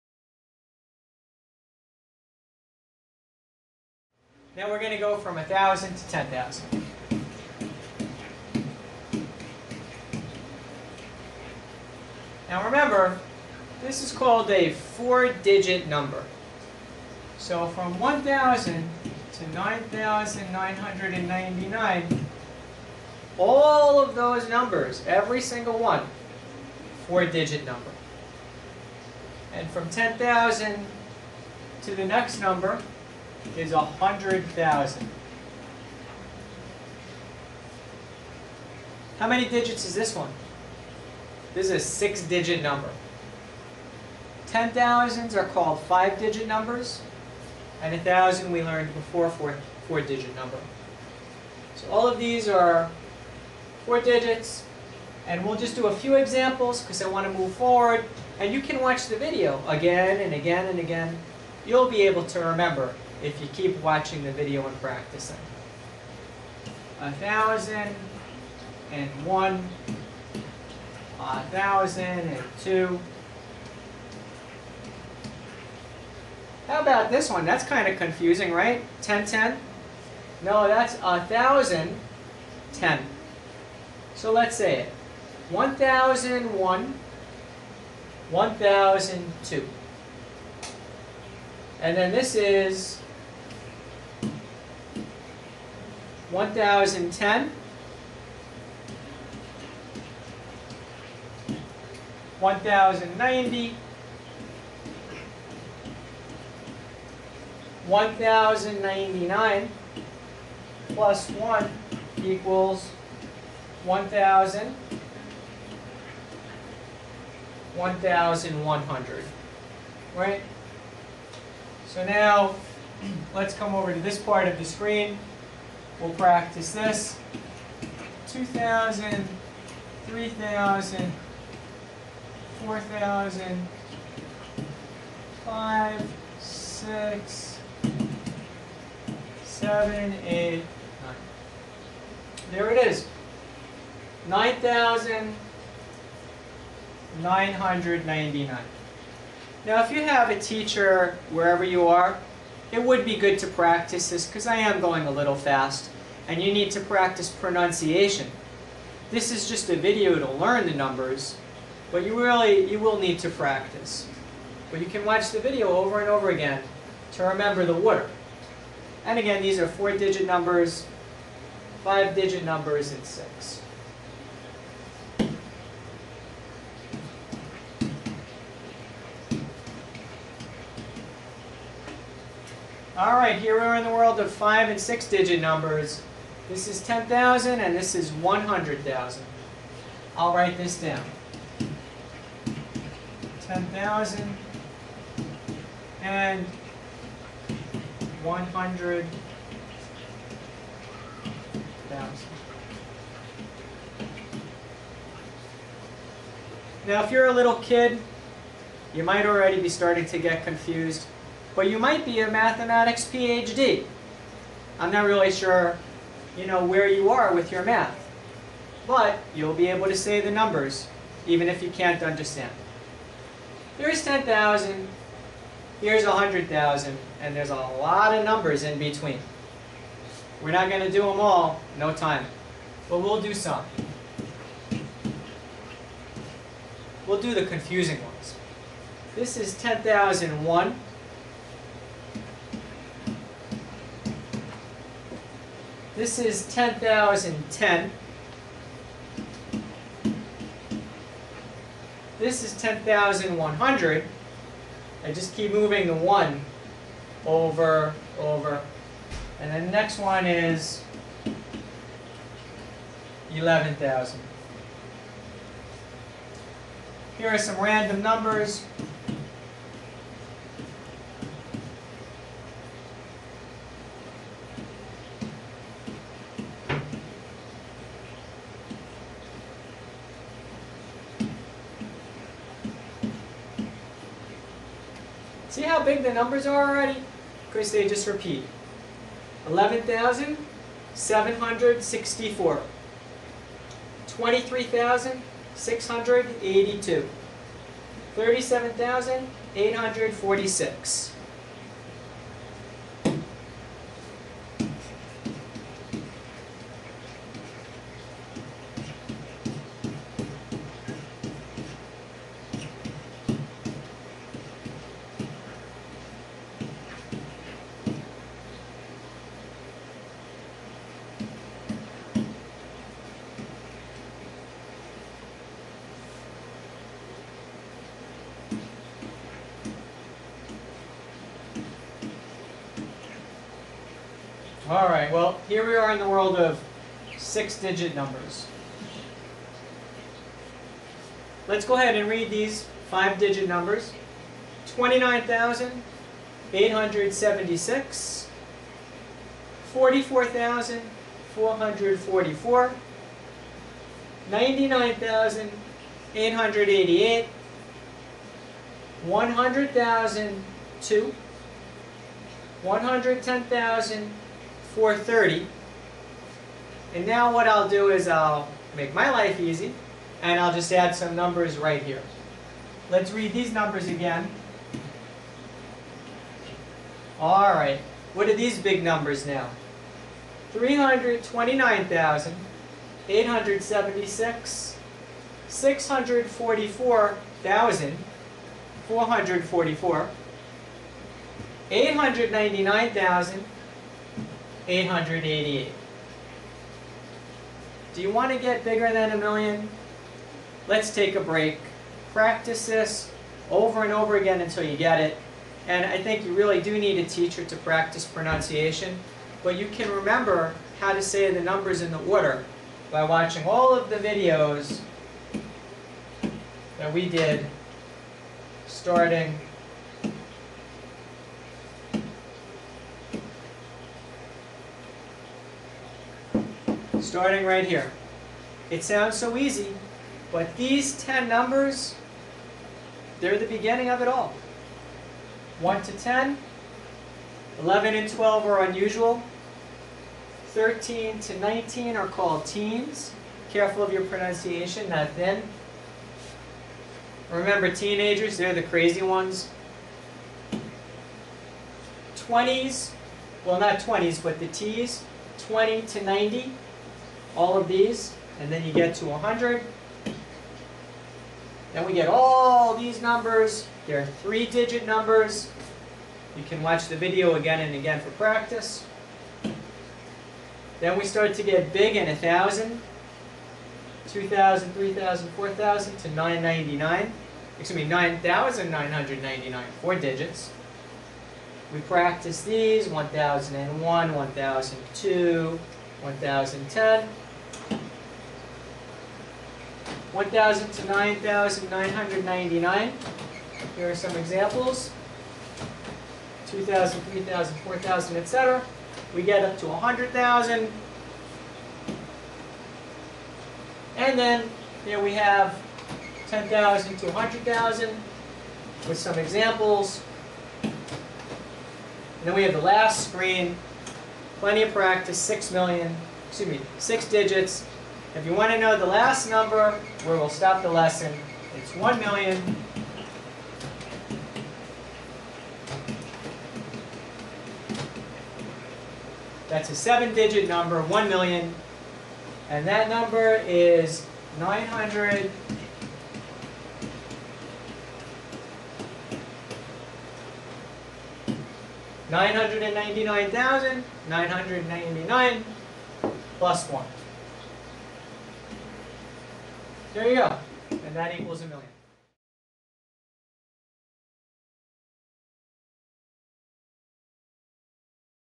Learn to say and Practice Numbers 1,000 - 1,000,000
Learn how to read and say all the numbers from 1,000 - 1,000,000. This accelerated method will help you to quickly use and understand how to say big numbers